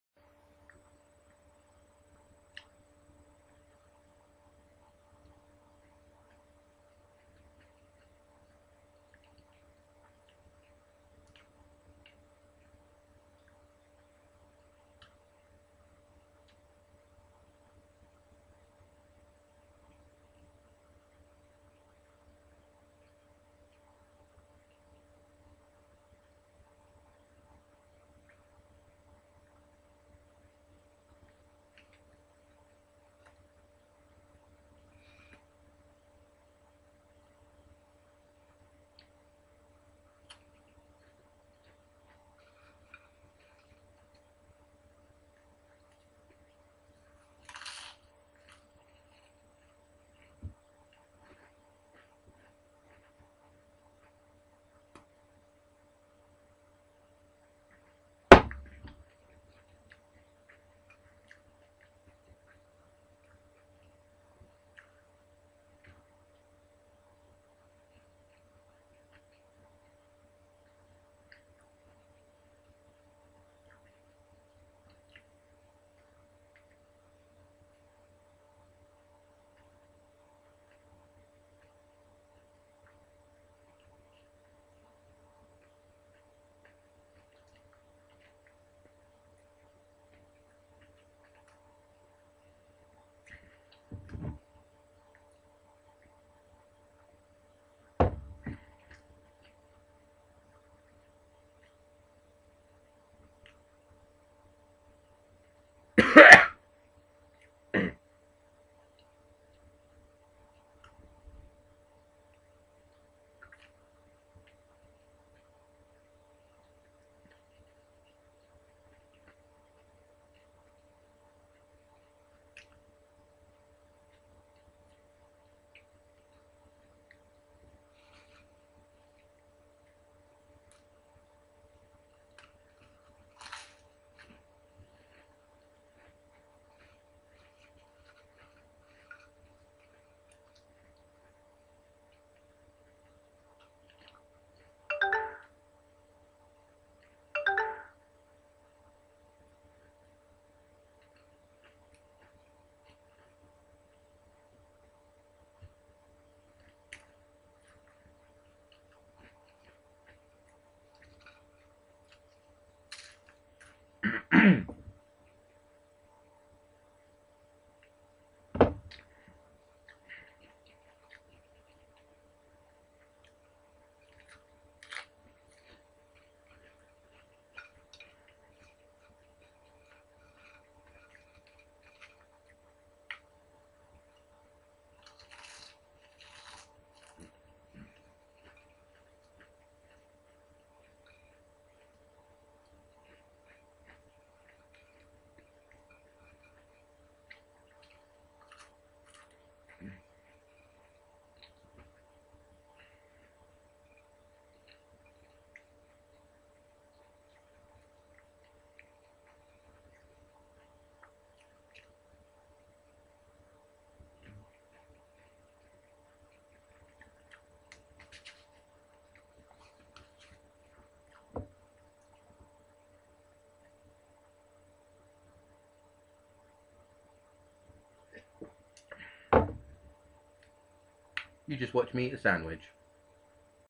Me eating a sandwhich :)
do i really need to explain this video clue is in the title. :L